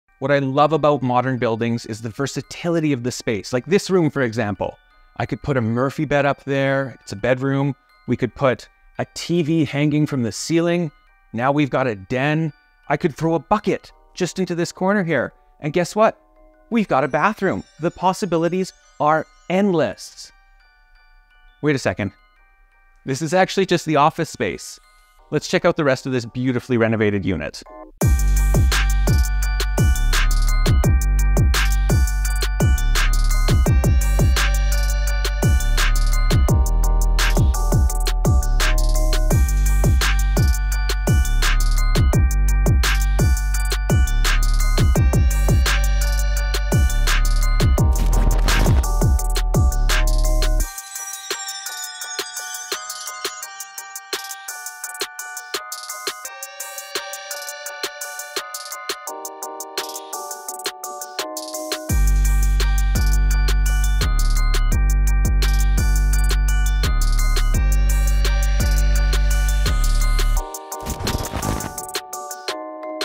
Fully Reonvated Home located in the Heart of Beltlline, Calgary AB!
Prepare to be blown away by this stunning 2 bedroom, 1 bathroom corner unit located in the heart of the Beltline. With 779 SqFt of fully renovated space and a distinctive separate office, this modern gem offers a perfect blend of style and functionality.
ㅤ
The kitchen boasts sleek stainless steel appliances, soft-close cabinetry, and exquisite quartz countertops with a waterfall edge, perfectly illuminated by under-cabinet lighting. The upgrades are evident throughout with laminate flooring that flows seamlessly, flat ceilings for a contemporary touch, a spacious walk-in closet in the primary bedroom, and custom Hunter Douglas blinds throughout. The secondary bedroom could easily accommodate a queen sized bed and has ample closet space. The best part about the separate office is that it allows you to actually utilize the second bedroom to the fullest potential, making this a uniquely practical unit!
ㅤ
Enjoy the outdoors on the expansive 152 square foot wraparound balcony – a delightful extension of your living space. Located within walking distance of 17th Ave, this residence offers easy access to Calgary's finest shops while maintaining a serene and private ambiance. Don't miss this opportunity to experience urban living at its finest in a prime Beltline location.
ㅤ
🔥COMING SOON🔥
ㅤ
📍 BELTLINE
📐 779 SQ FT
🛌 2 BEDS
🚿 1 BATHS
ㅤ
📥 For showings and information: